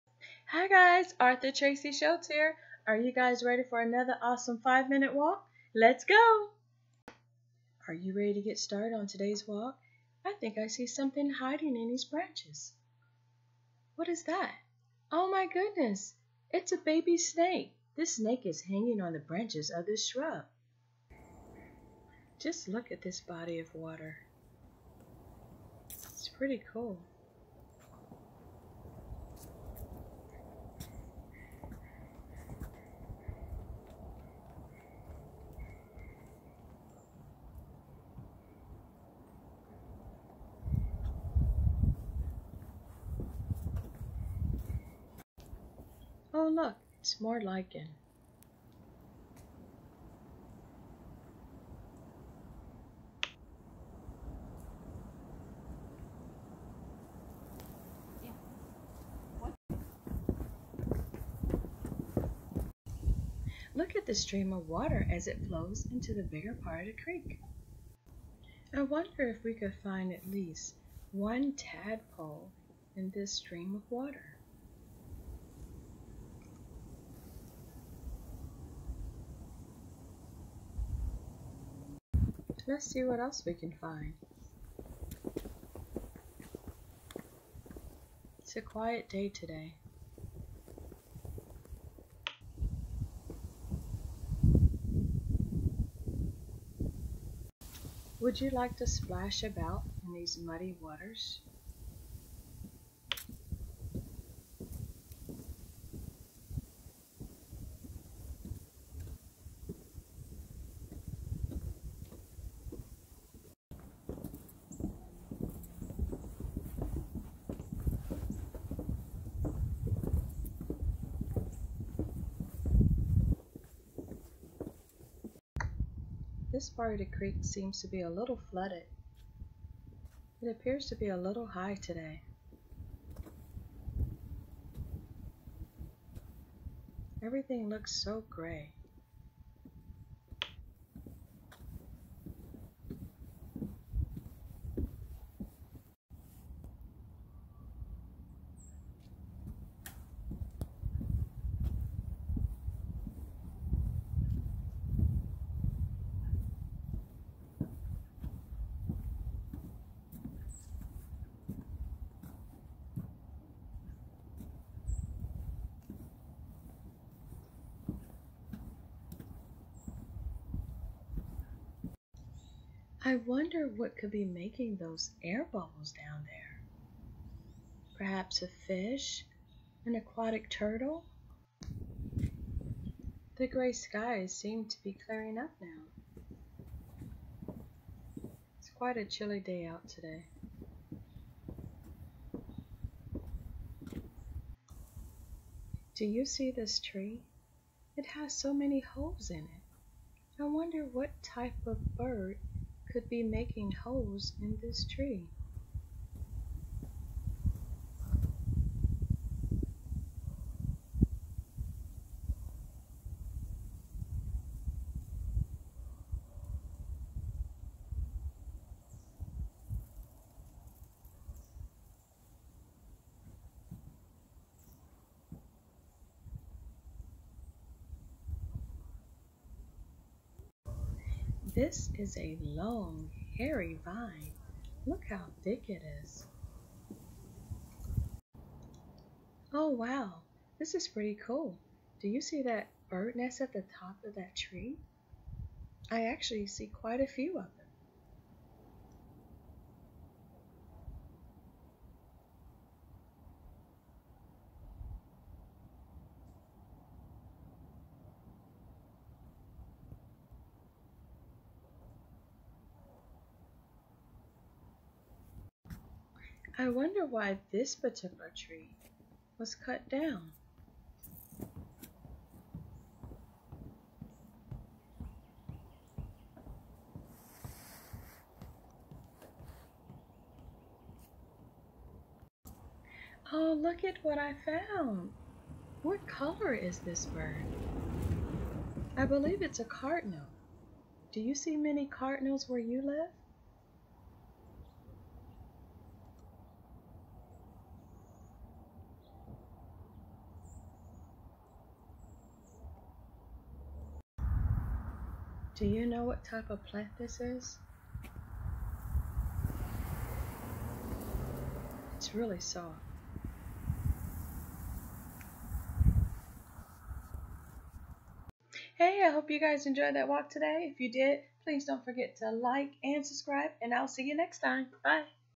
5 Min Nature Walk
Nature walk. Nature Walk with Children's Author Tracie Choates.